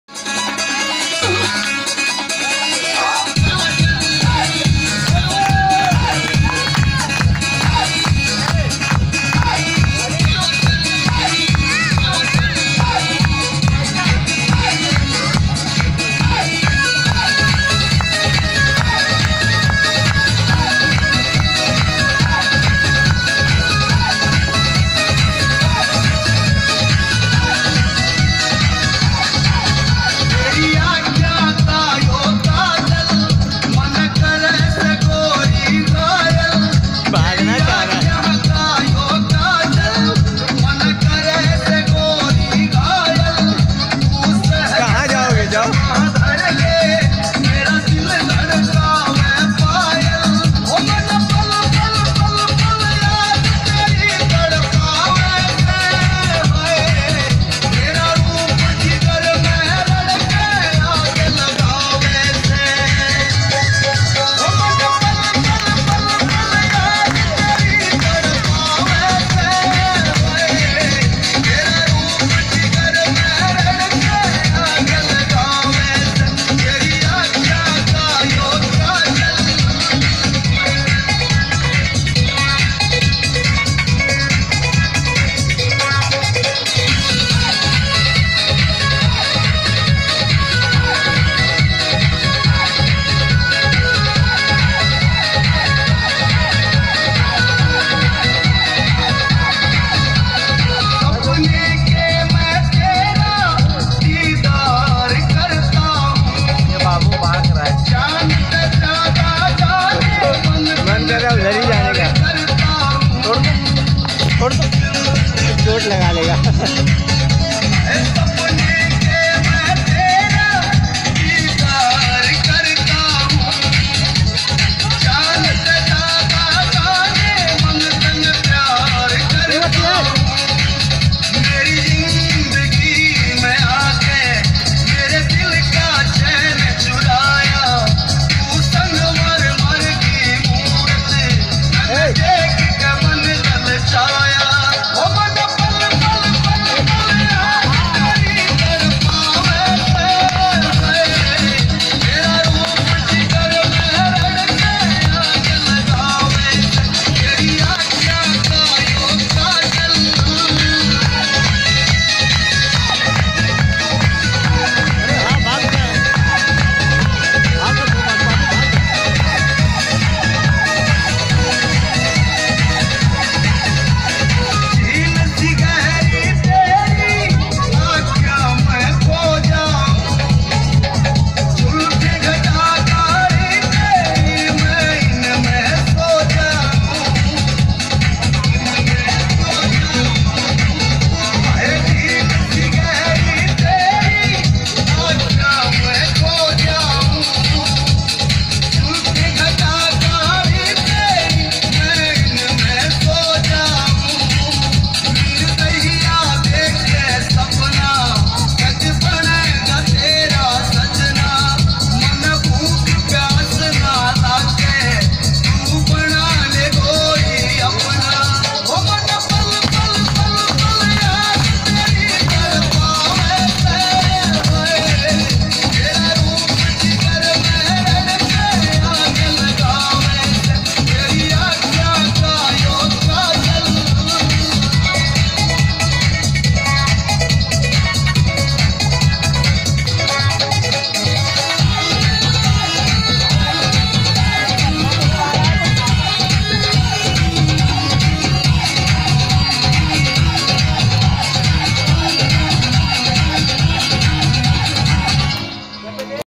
बड़ी बहन की शादी मे छोटी बहन का सपना चौधरी को चुनौती वाला ड़ांस।
आपके सामने एक 9 साल की बच्ची का एनर्जेटिक डांस